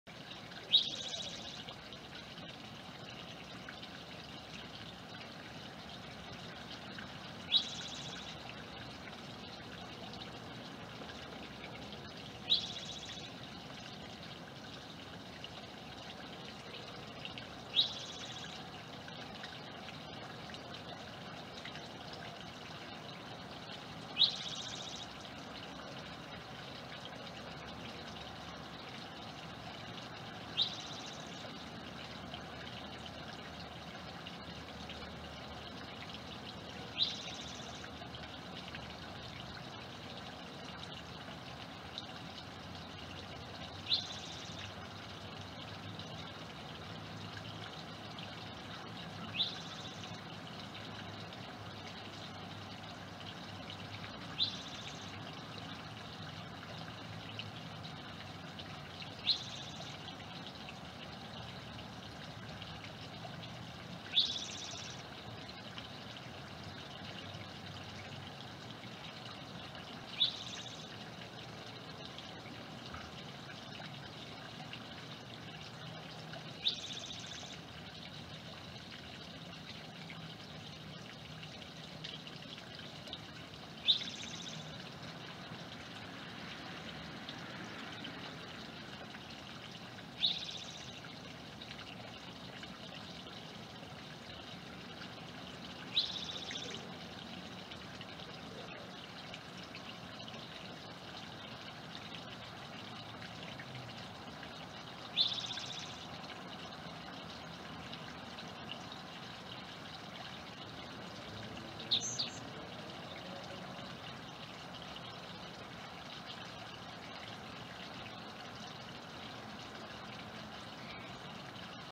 The Songs of Spring is Coming! (Sound)
Not sure what bird this is, may be a Song Sparrow, but is the first one I hard this year kicking off Spring!